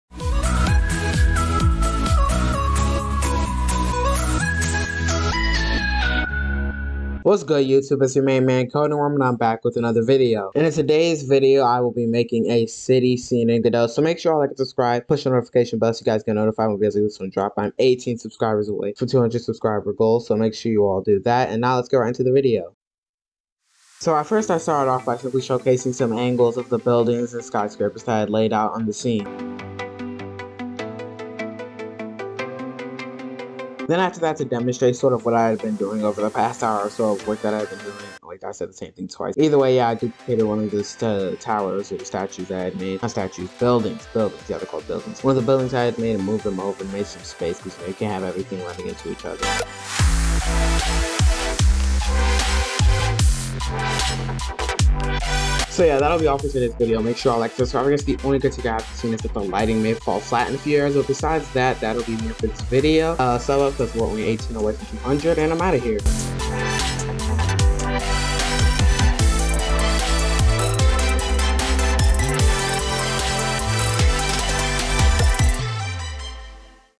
Making A City in Godot
I have had this one in the back of my mind for a while now but simply never chose to do it. Don't why but it is here. Make sure you sub me up so I can get 200. So yeah, enjoy the video and your day. Peace Out.